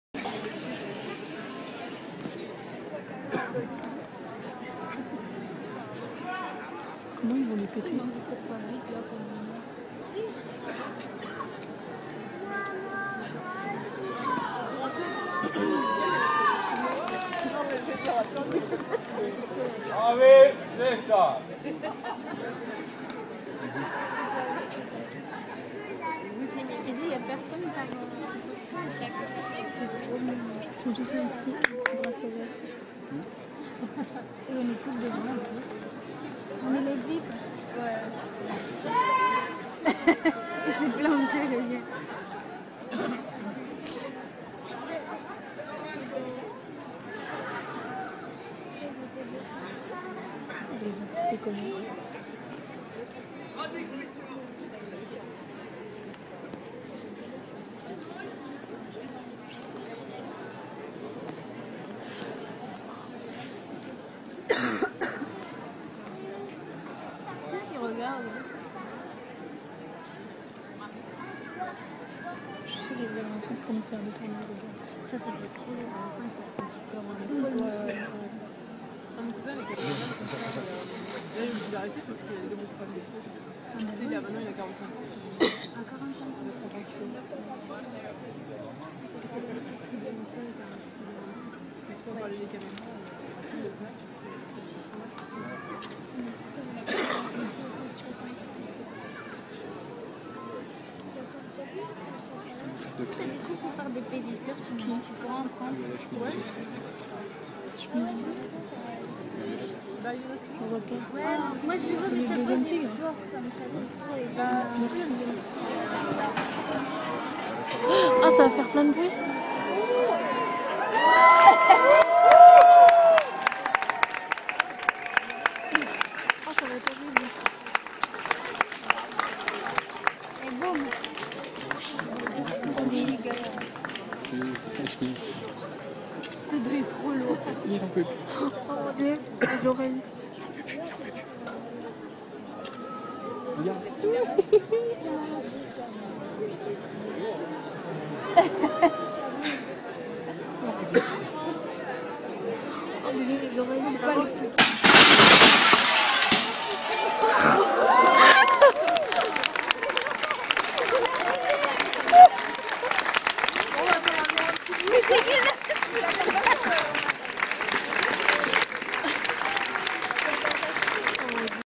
Tarzan Noir, performance de Marcello Maloberti - Nuit blanche Paris 2011
Tarzan Noir
Co-commissariat et organisation
Pierre Bal-Blanc, directeur CAC Brétigny
Luca Cerizza, commissaire indépendant
Julien Duc-Maugé, commissaire projet culturel CAC Brétigny
Martine Brigandat, directrice centre social La Fontaine
Mohamed Bouazzaoui, Animateur centre social La Fontaine
Alexia Fabre et Frank Lamy, directeurs artistique de la Nuit Blanche Paris 2011